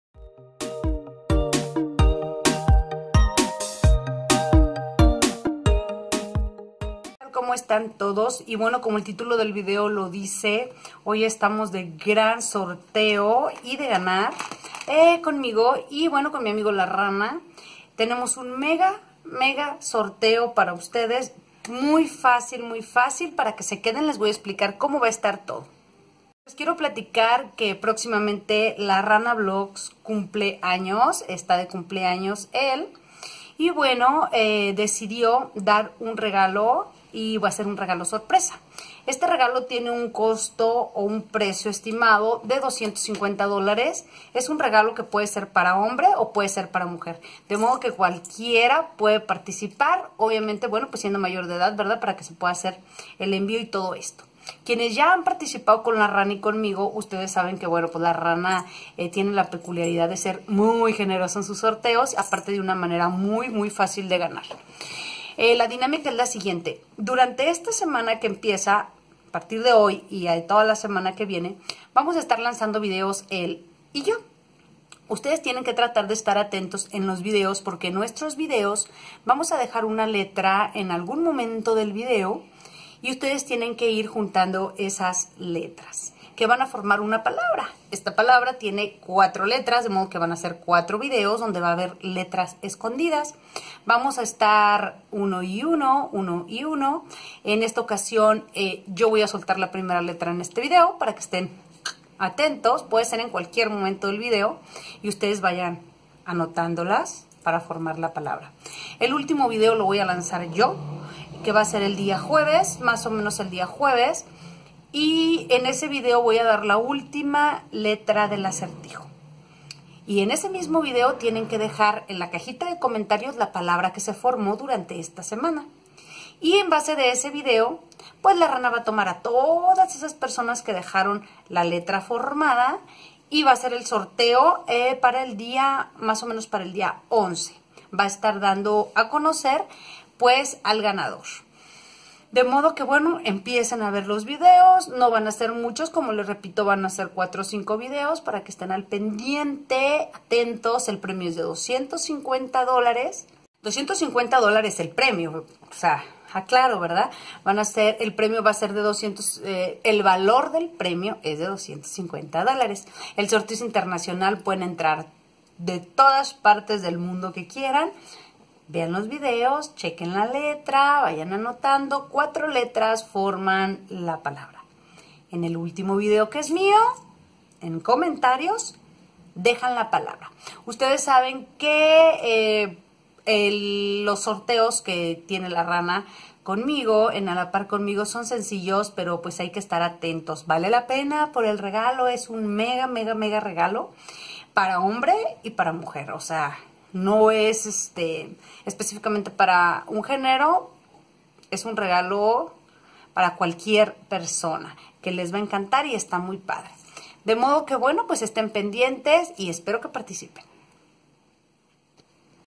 SORTEO! GANA CON LIZZ Y LA RANA!////CERRADO
Hoy estamos de sorteo en Honor al cumpleaños de la Rana
LINK DEL CANAL DE LA RANA:
DINAMICA DEL SORTEO:
Durante toda esta semana que comienza, contando este video, apareceran letras en los videos mios y de la Rana, tendras que ir anotandolas para descubrir la palabra misteriosa, la cual consta de cuatro letras, el dia jueves yo subire el ultimo video con la ultima letra y en ese video, en los comentarios tendras que poner la palabra que se formo y con eso estaras participando por un premio con valor de 250 dolares, el sorteo es Internacional y los resultados se daran el dia 11 de Julio en el canal de la Rana.